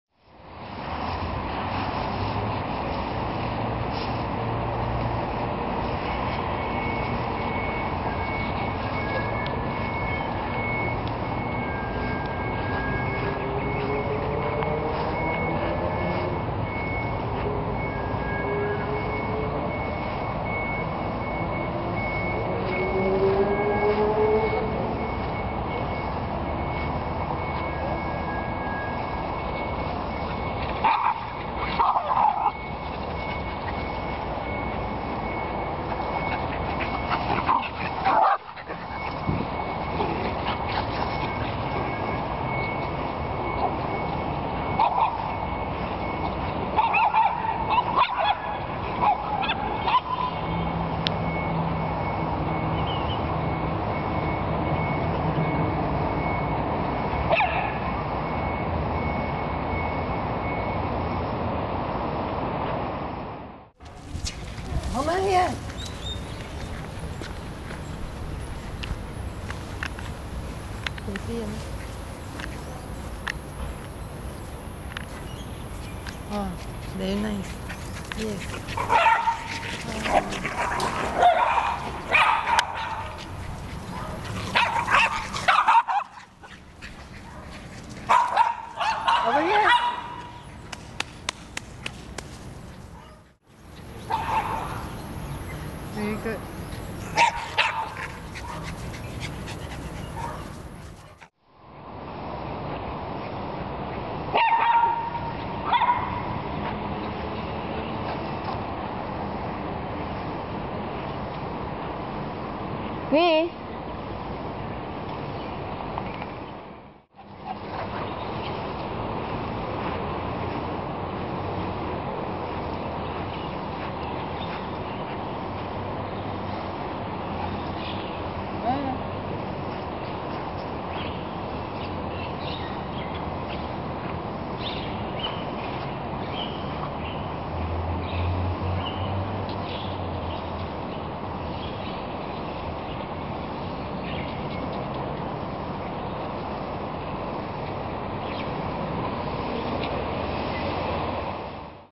My 3 mini schnauzers having fun RUNNING FREE IN THE SUN!
This video is a lookback at my three mini schnauzers younger days in the sun. We would bring them to this huge field very early every Sunday morning, (when there were hardly anyone around), to give them a chance to stretch their legs & have fun running free.
(We could only let them free cos they had reliable recalls)